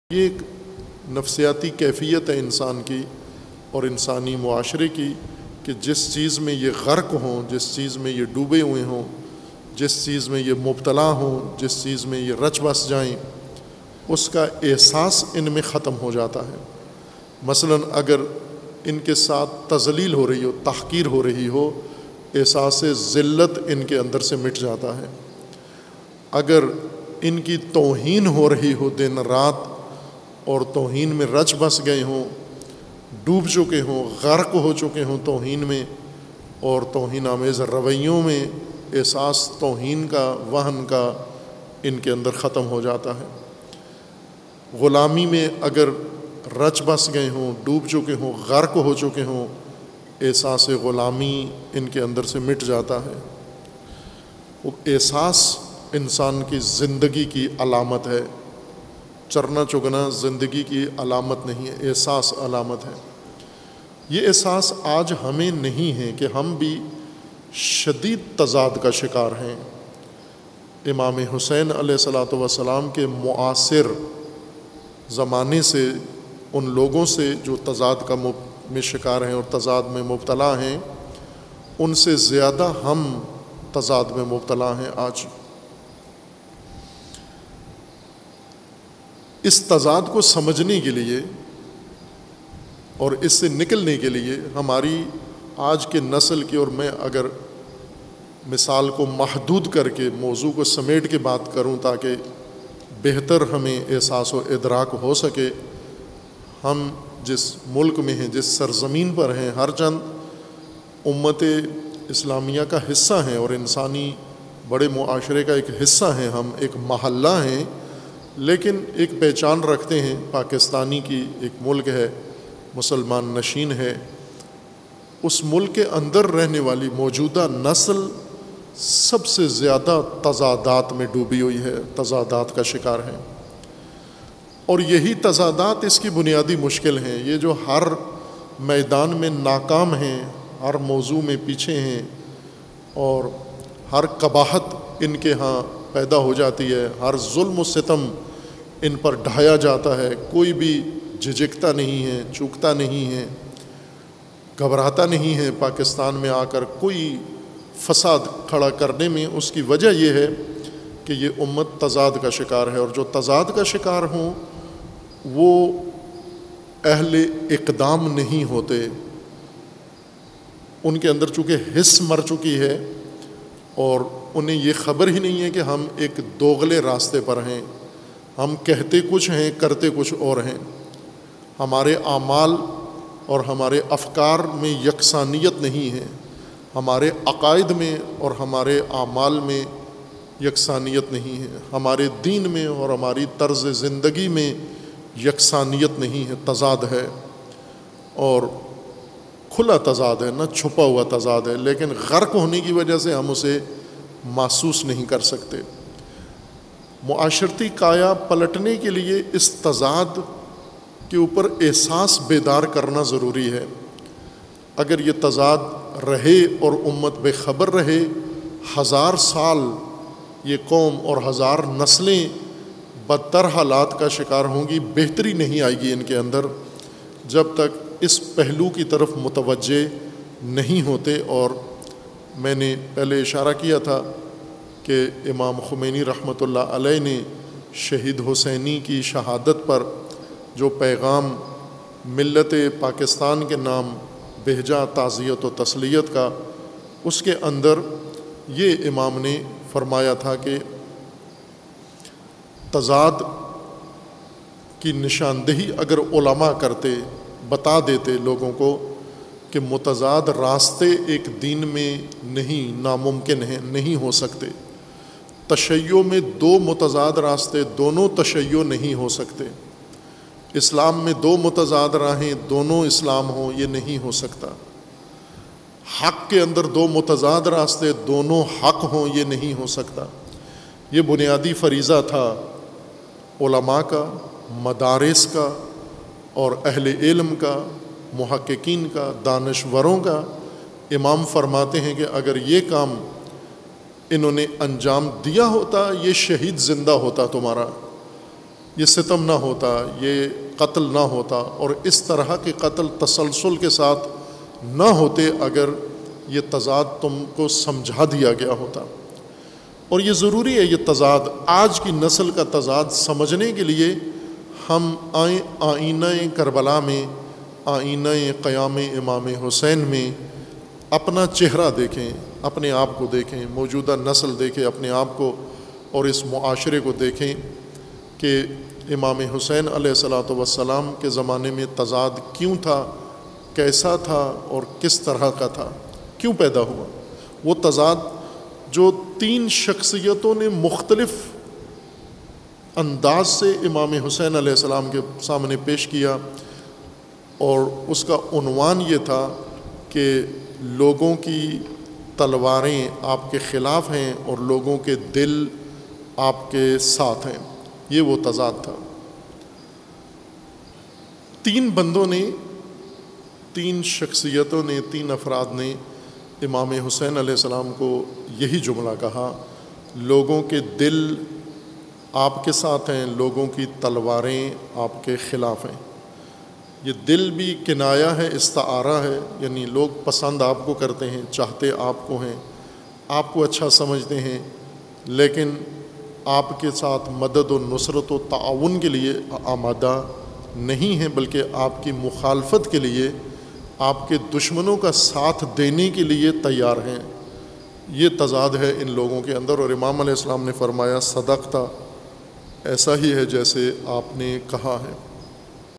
آج کی نسل کا تضاد، کربلا کے آئینہ میں | Agha Syed Jawad Naqvi | Clip-6th Majlis Muharram 1444
6th Muharram ul haram 1444
5th August 2022